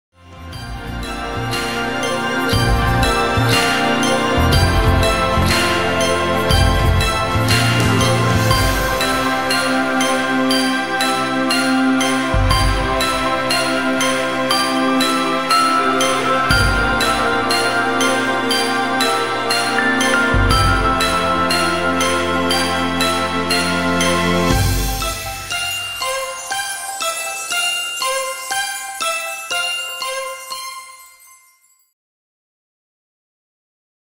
DIY toilet paper origami
How to make toilet paper candle origami. / Kynttiläorigami wc-paperirullaan.